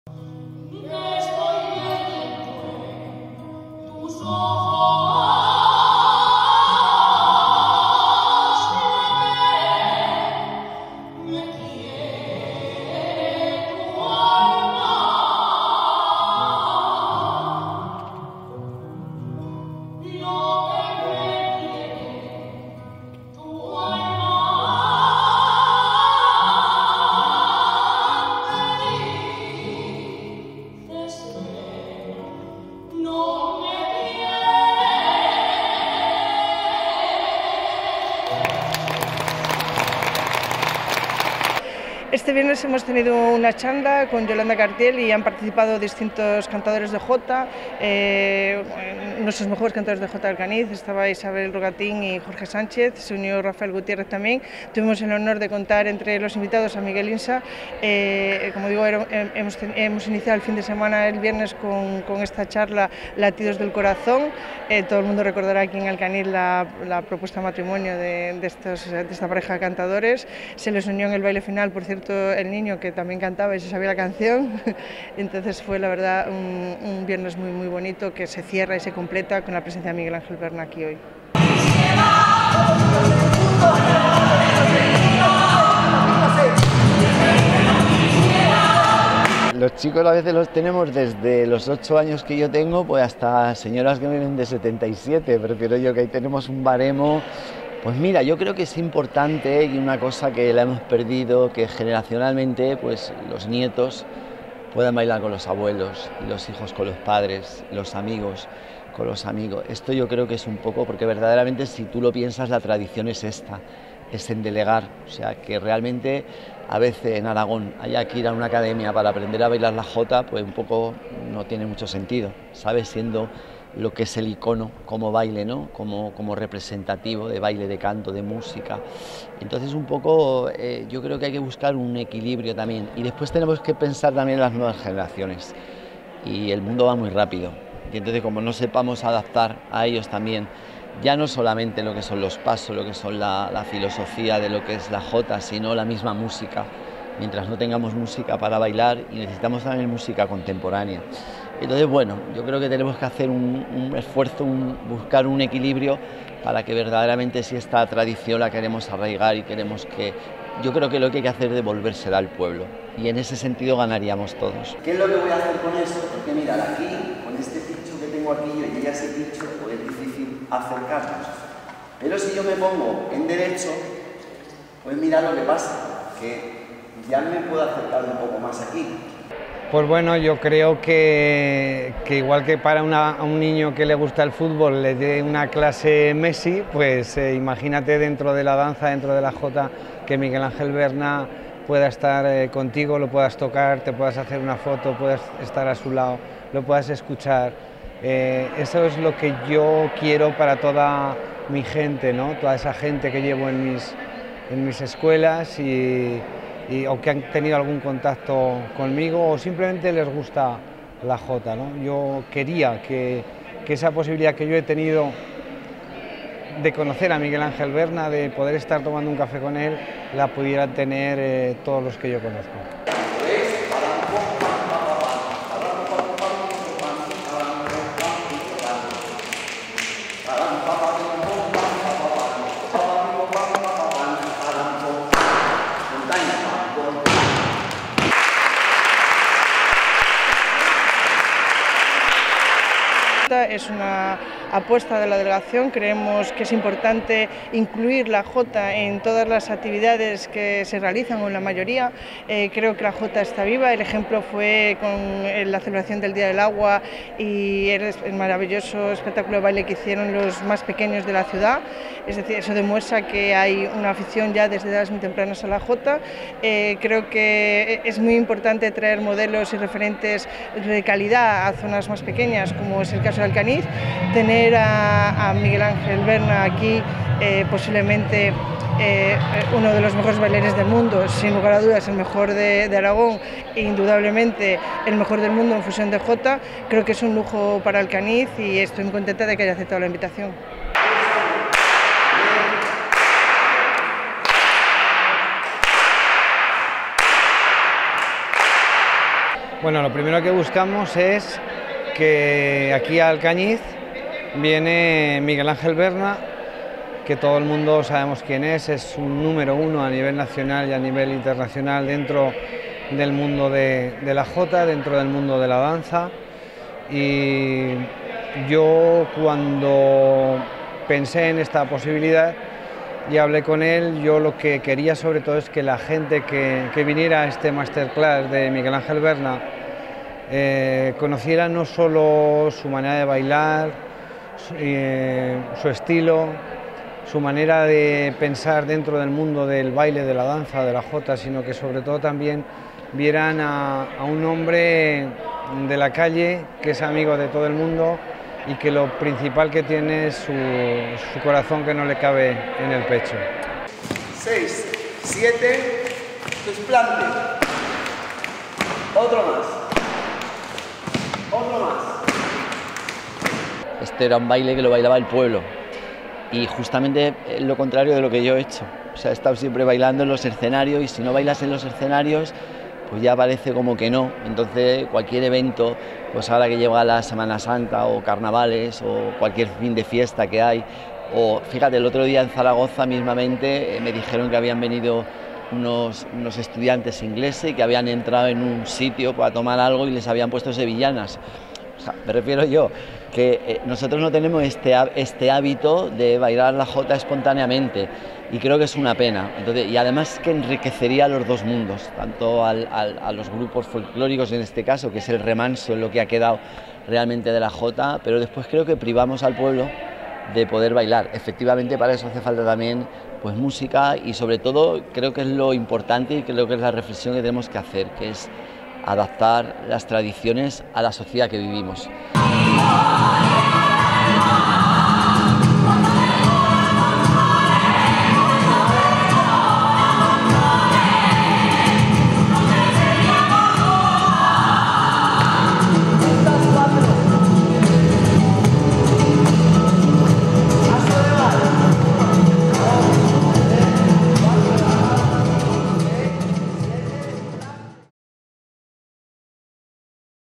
Masterclass de Miguel Ángel Berna en Alcañiz
El área de Infancia y Juventud de Alcañiz, dirigido por Susana Mene, organizó para la mañana del sábado 25 de marzo de 2023 una masterclass de jota bailada impartida por Miguel Ángel Berna. Los actos comenzaron el viernes con jotas en el centro joven y ha terminado con Berna enseñando a varias decenas de alumnos en el Centro Joven de Alcañiz. Tanto Mene, como concejal de Infancia y Juventud, como José Antonio Pedrós, profesor de baile de jota, se mostraron muy satisfechos con la inscripción para el curso y, sobre todo, con la oportunidad de tener en Alcañiz al mejor bailarín de jota que tiene Aragón.